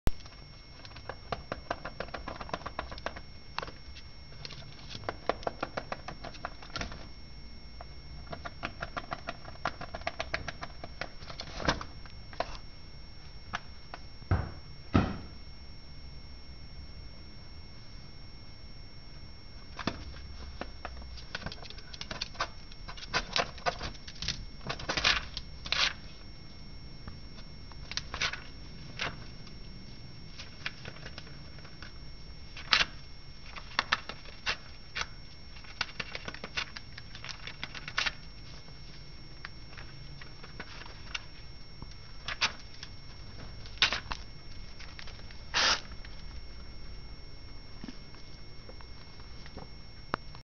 Rabbit & Guinea Pig Eating a Book
it's not eating~ they're biting the paper i gave because their teeth are 'itchy'
Remember rodent teeth grow continuously throughout their lives?